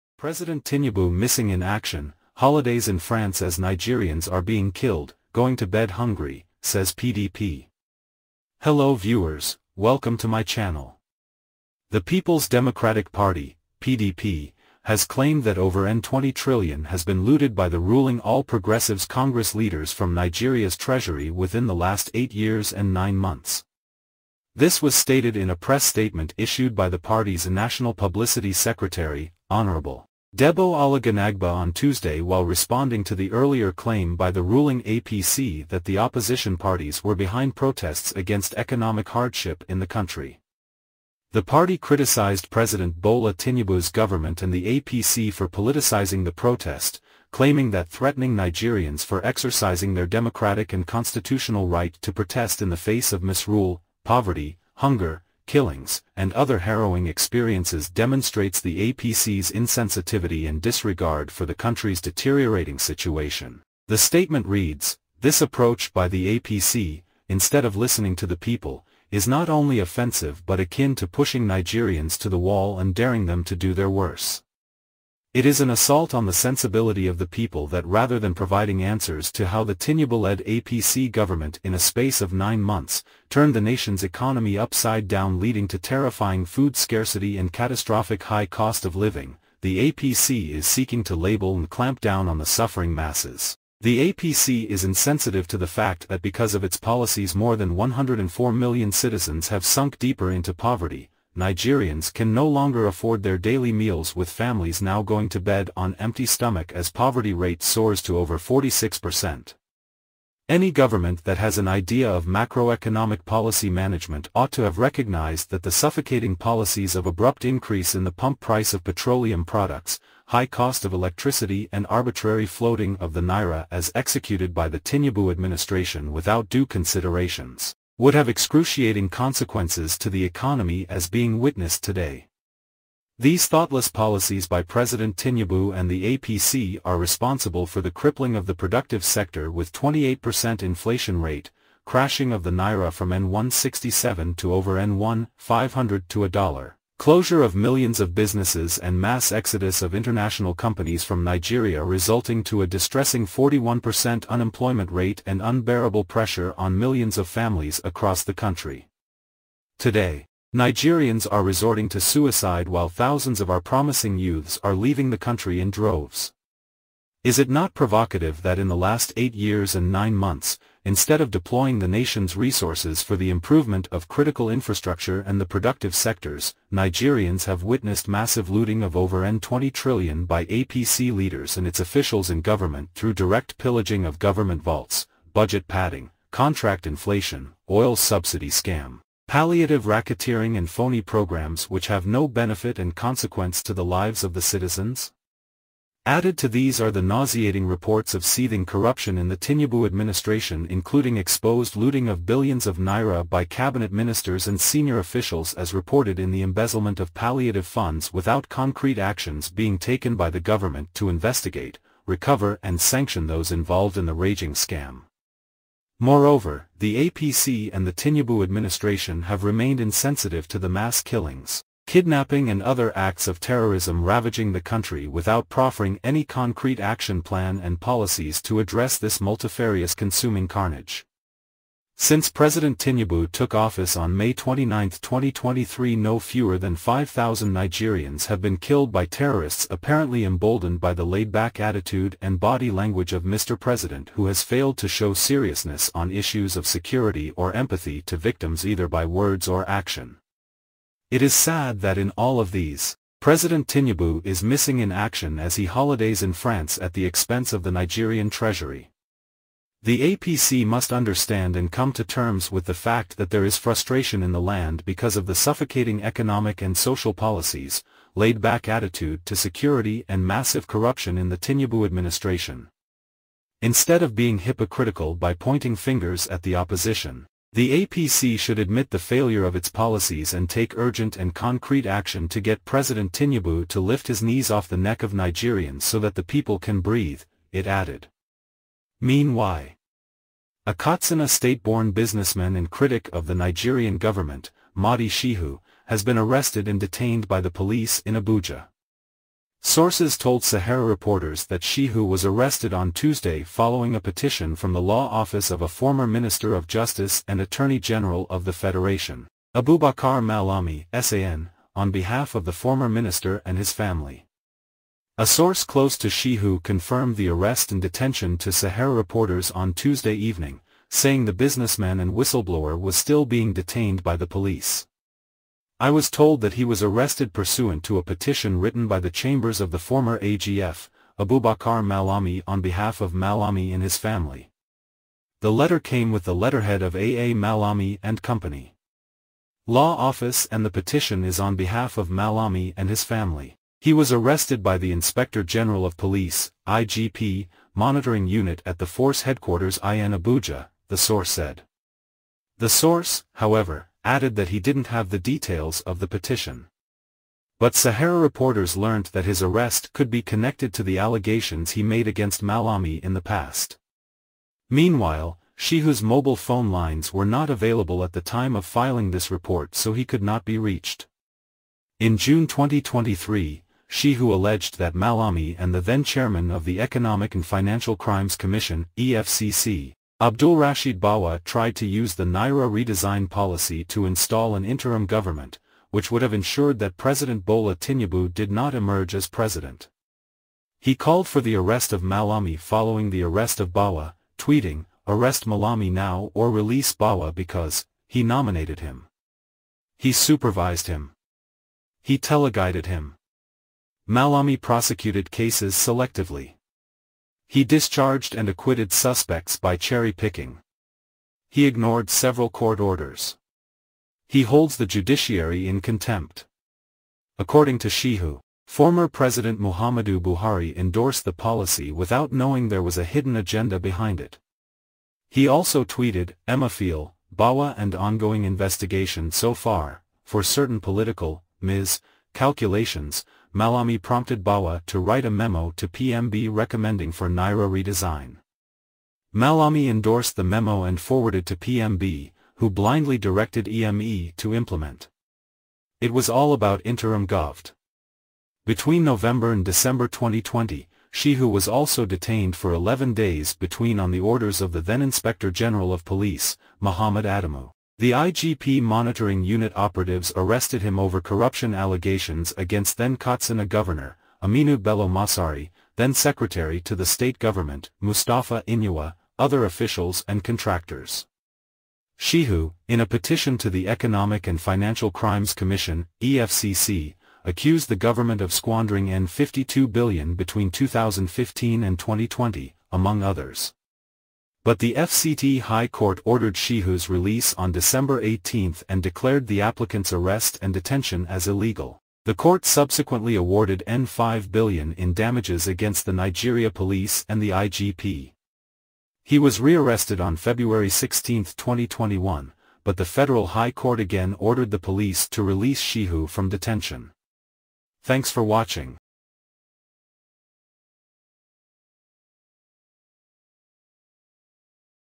Tinubu Is A Failure, Holidays In France As Nigerians Are Being Killed, Going To Bed Hungry, US GOVT
ALL NEWS AND MEDIA IS A 24H GENERAL ENTERTAINMENT CHANNEL, WE CREATE AND UPLOAD ALL KIND OF GREAT NEWS AND VIDEOS FOR YOUR VIEW